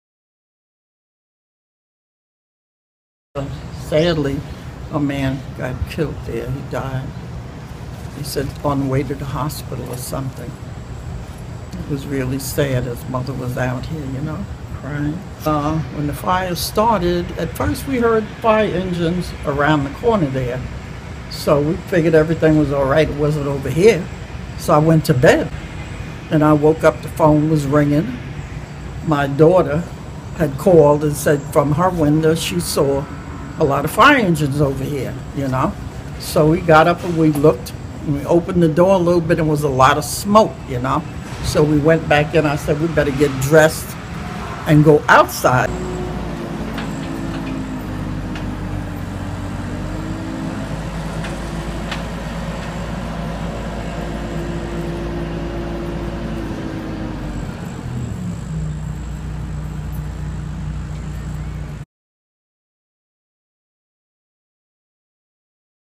Caleb Wilder dies after saving residents in Mariners Harbor fire
Police credit Caleb Wilder, 37, with saving residents from a fire that claimed his life on Sunday, Oct. 11, 2015, at 124 Brabant St. in the Mariners Harbor Houses. Wilder’s mother, Gwen, lived in the second-floor apartment where the fire allegedly st